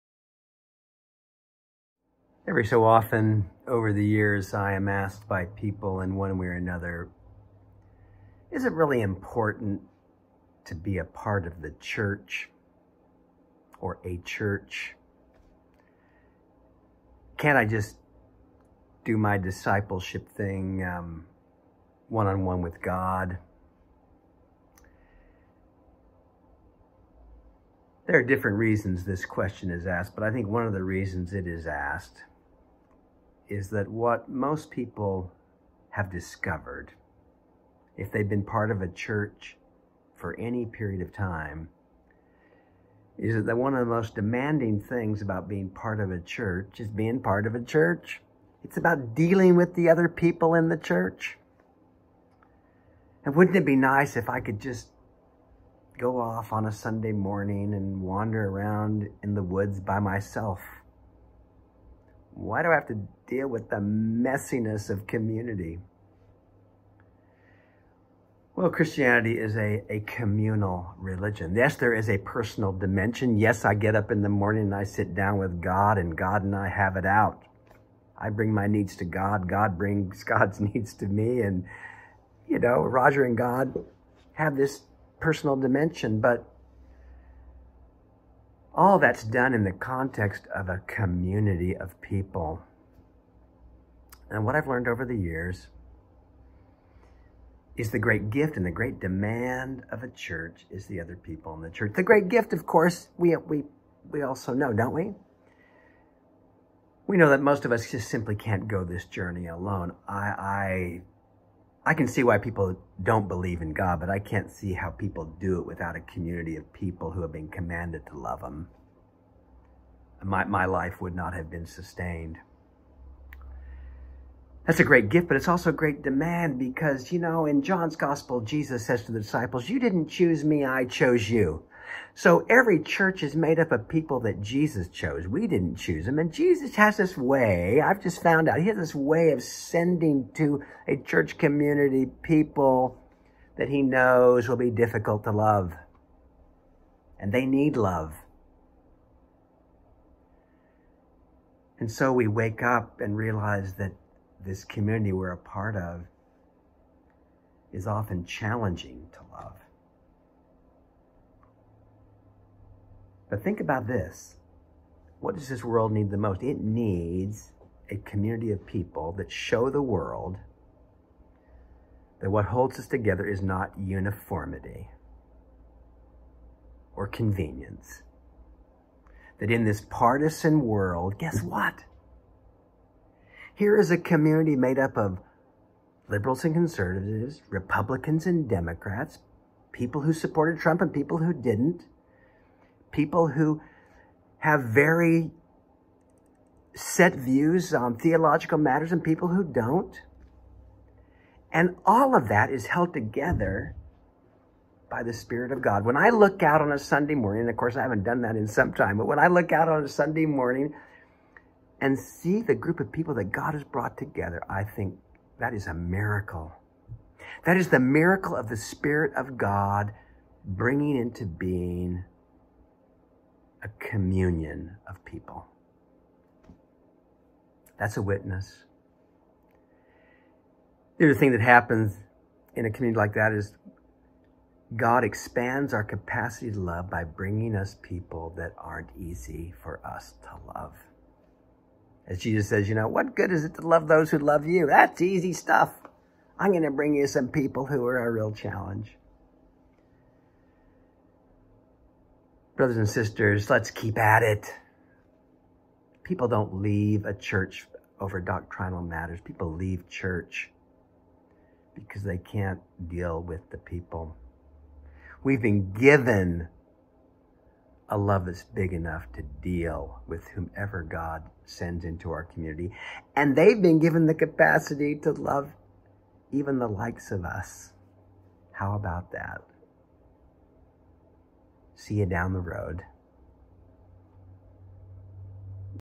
The Challenge of Being Part of a Church
NO HOLDS BARRED - Full disclosure with God.

The January 29, 2021 vlog from St. Timothy's Rector, Roger Greene.

For more information about the mission of Saint Timothy's find us on the web